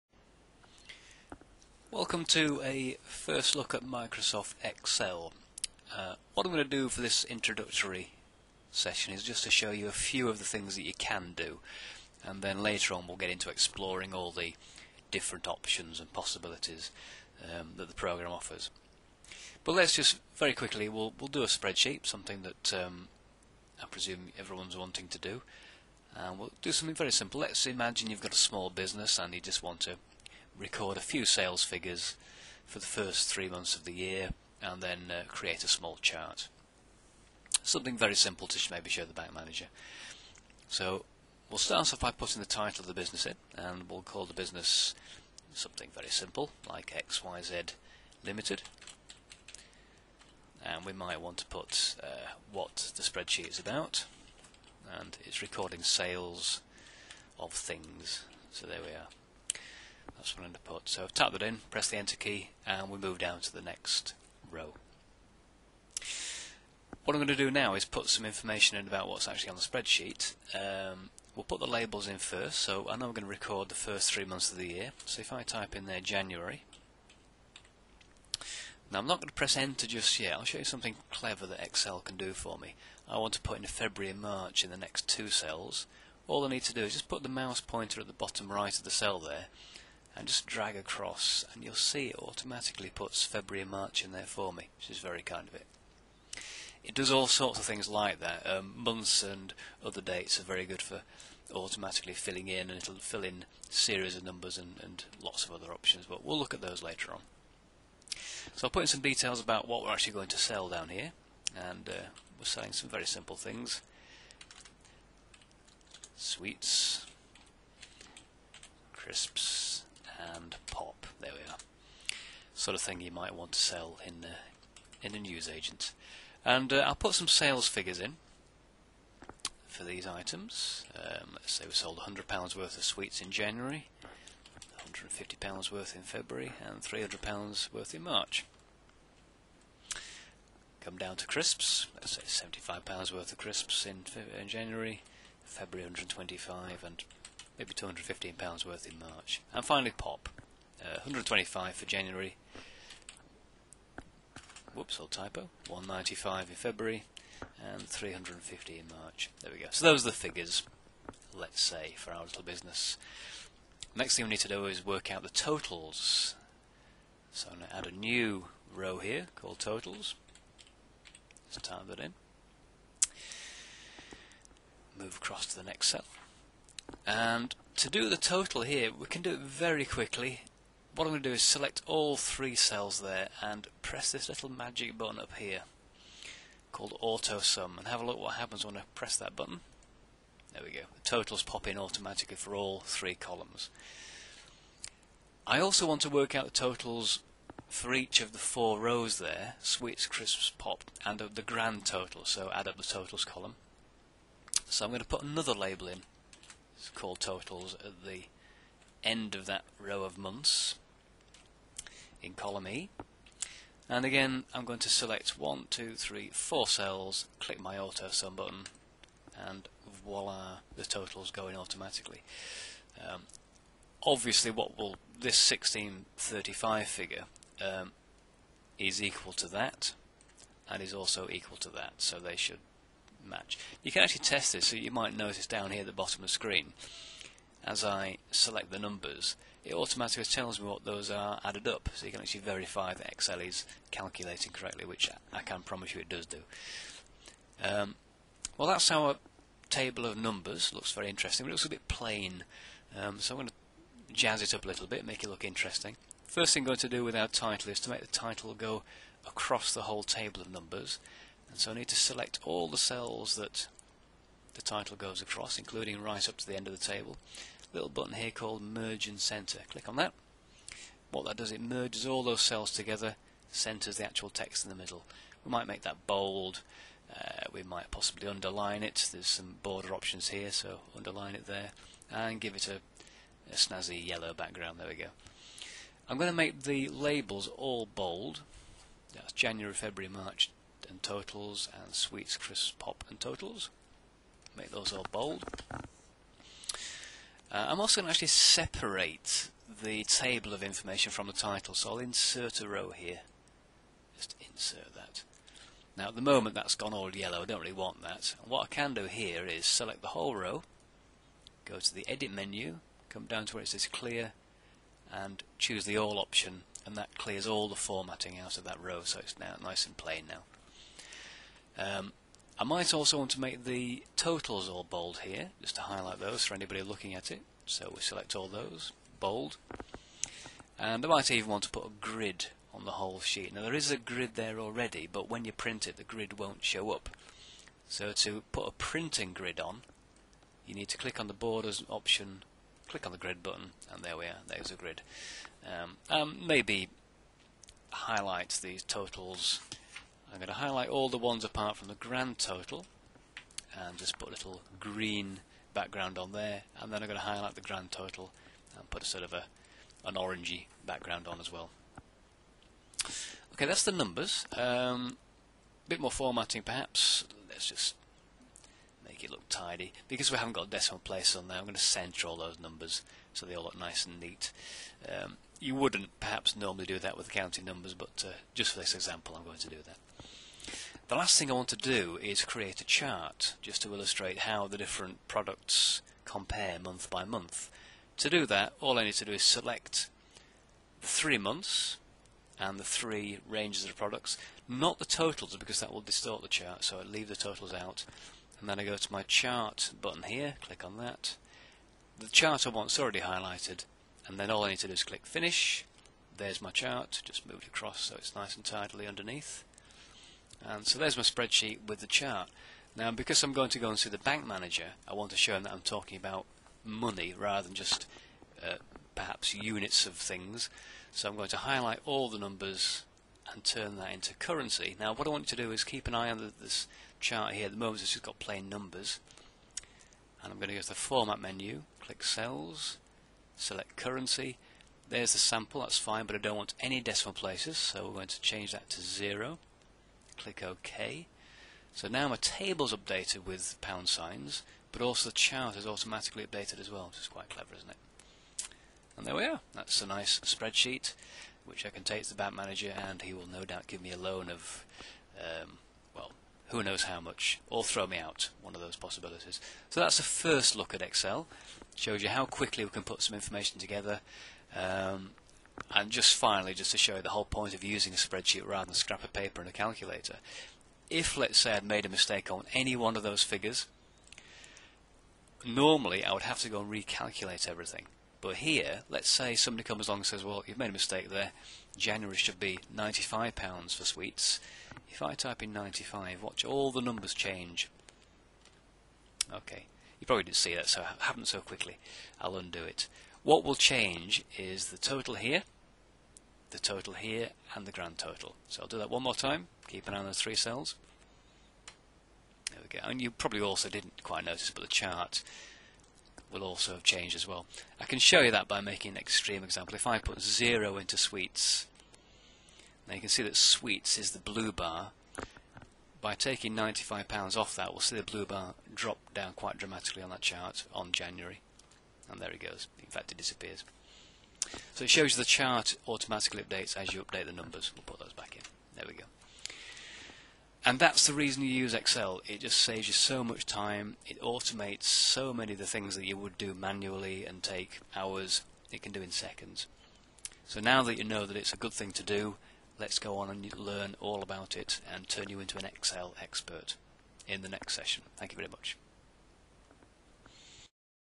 Microsoft Excel Tutorial for Beginners #1 - Overview
This tutorial:   A quick look at setting up a simple spreadsheet in Excel complete with a chart.  More tutorials to follow that will go into more detail on how to use the different features of MS Excel.